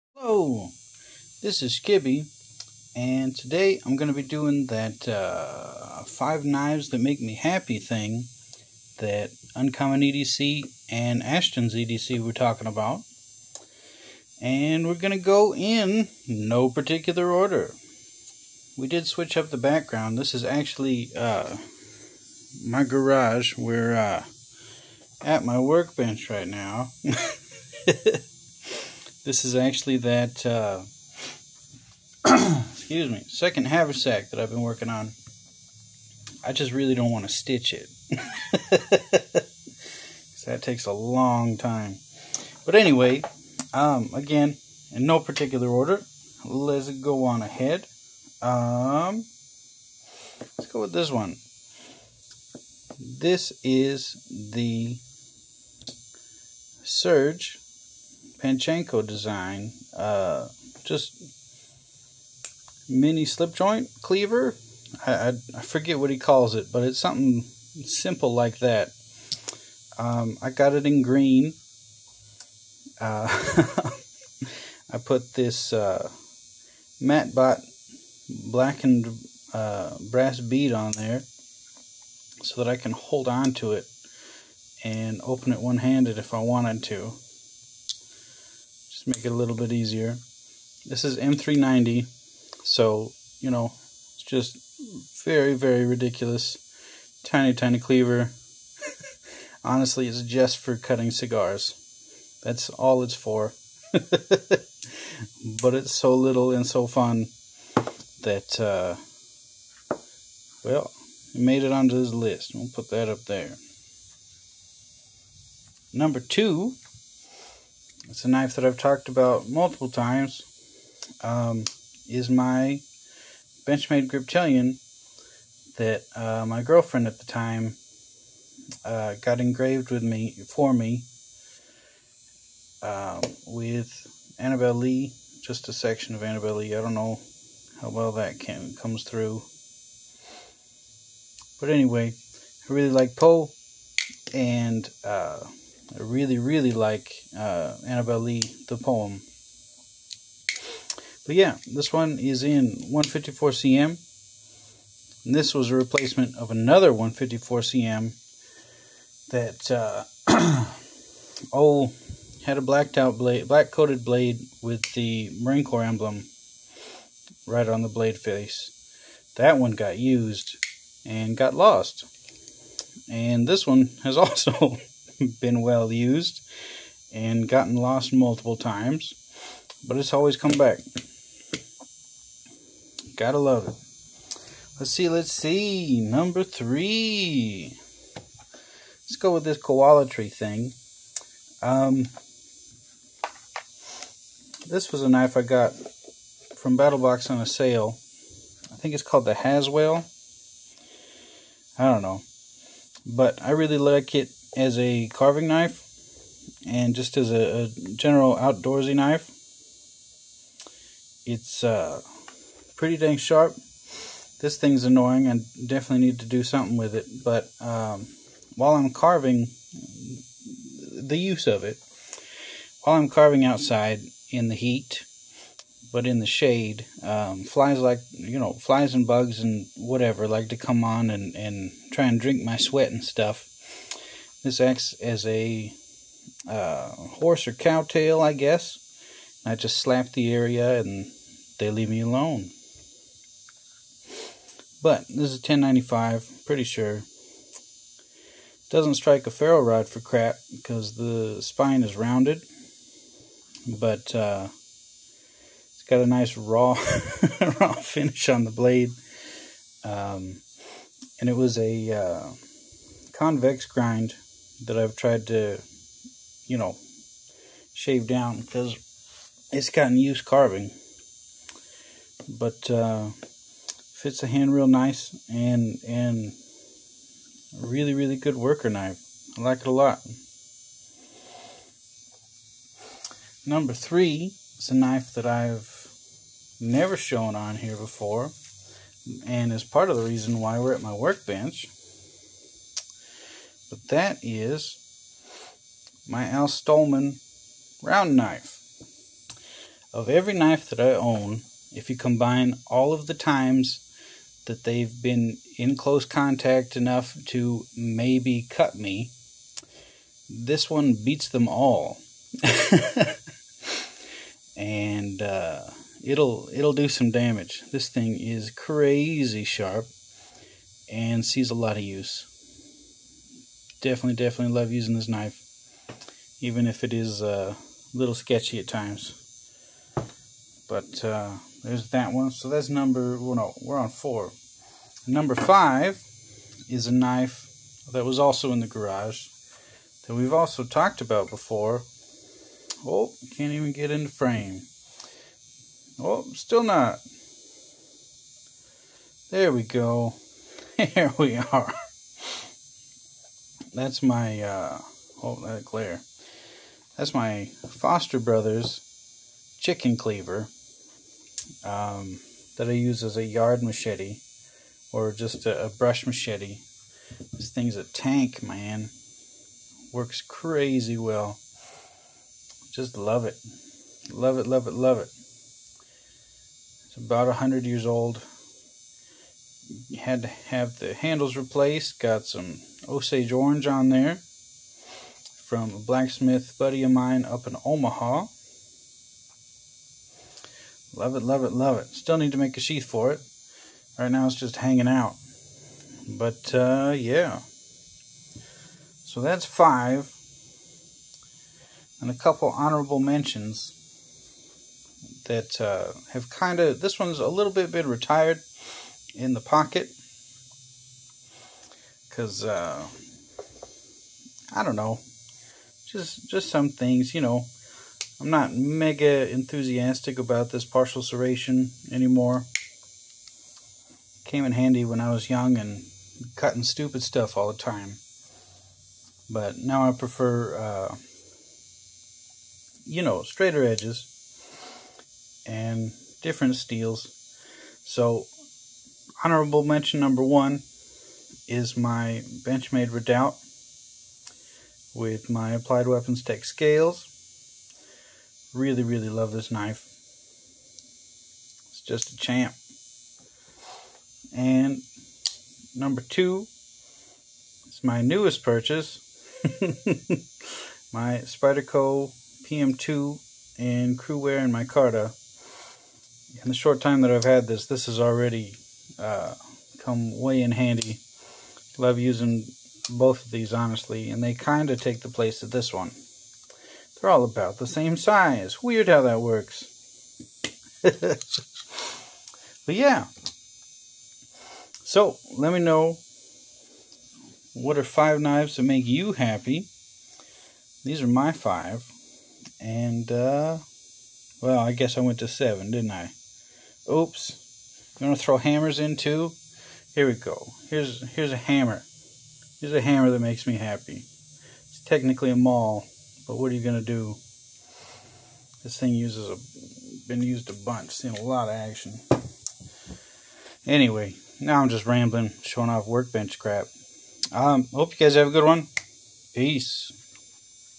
Five Knives That Make Me Happy!
GEEZ-OW! I need to figure out this new camera's quirks... cuz that was NOT the frame on the viewfinder and dunno why it cut out the first few seconds... oof...

Anyhow, Five (well... 7...) knives that make me happy!

What are yours?

Shout-out to @Ashton.everydaycarry  for inspiring me to do this and @UncommonEDC for inspiring him!